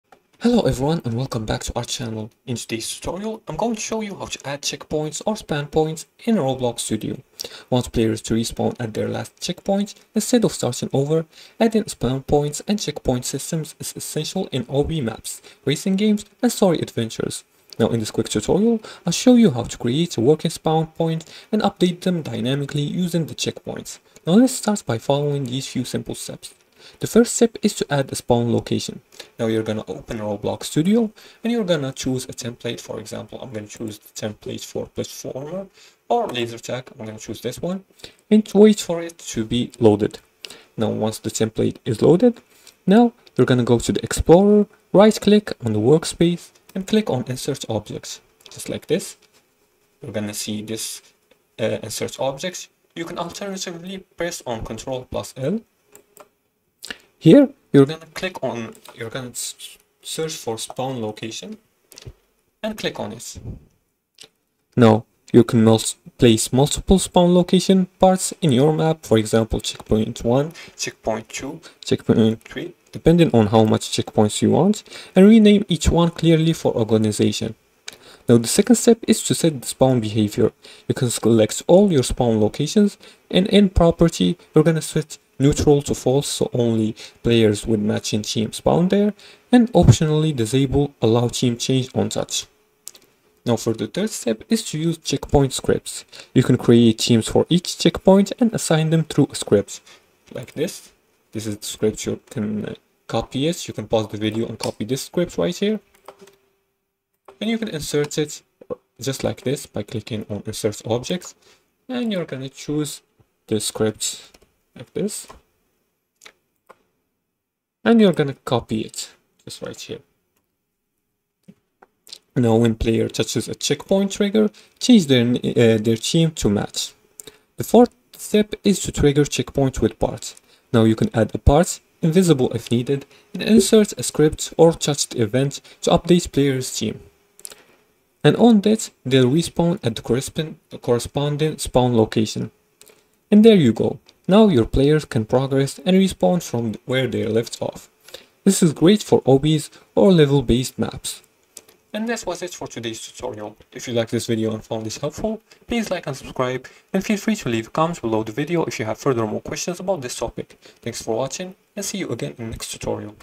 How To Add Checkpoints Spawn Points in Roblox Studio 2025! Tutorial
Today we talk about add checkpoints spawn points in roblox studio,roblox studio,roblox tutorial,roblox studio tutorial